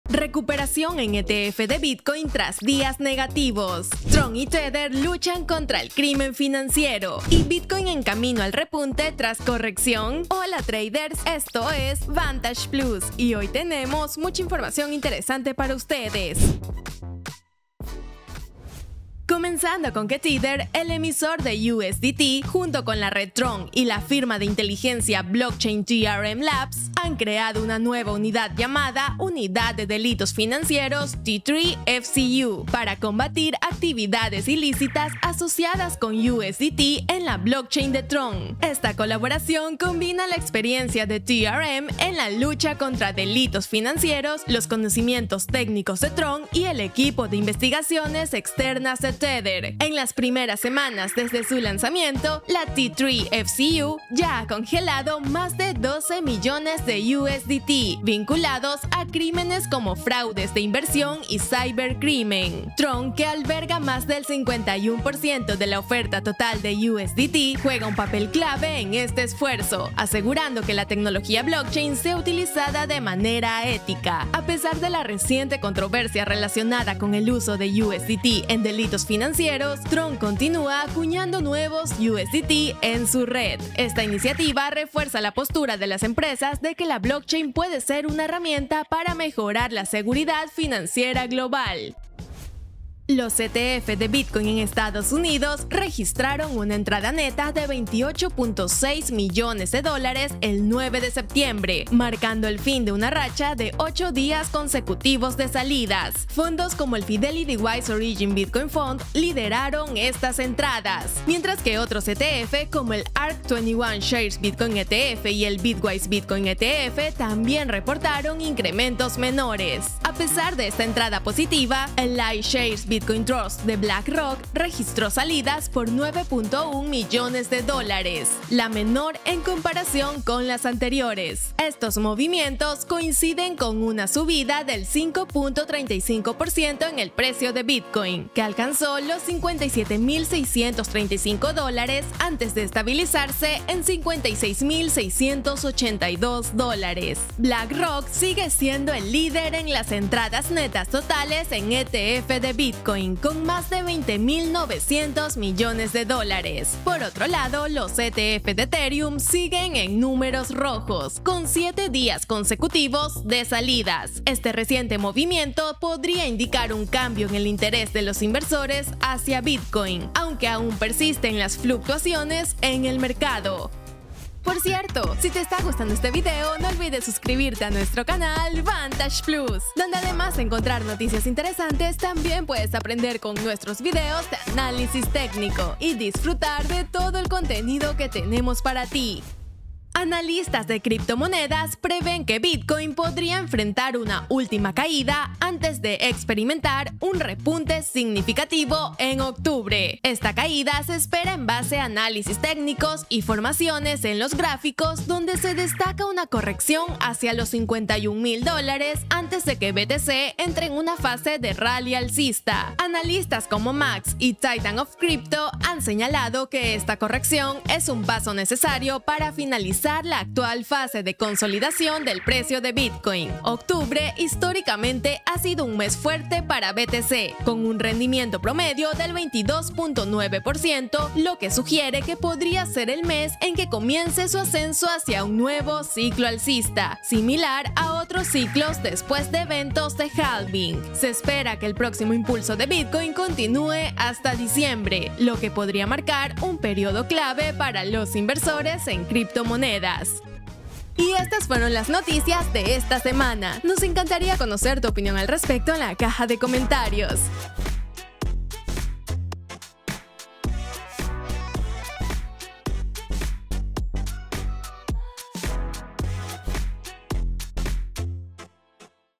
✅ Noticias Cripto: ¿BTC listo para repuntar? | USDT contra el crimen | ETF de BTC se recupera
- Tron y Tether luchan contra el crimen financiero
- Bitcoin: ¿camino al repunte tras corrección?
- Recuperación en ETF de BTC tras días negativos